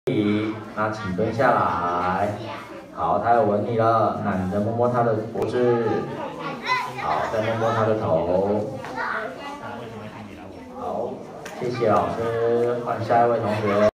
台中中坑國小《獨一無二的家人》校園巡迴生命教育片段
她叫小米，上山下海，有著令人望塵莫及的旅遊及校園巡迴紀錄。推動寵物友善環境，將飼主責任為核心的生命教育傳達給孩童，學校稱呼她為小米小姐，是台灣史上第一個巡迴過6座外島及其離島校園、巡迴次數破百場的旅犬。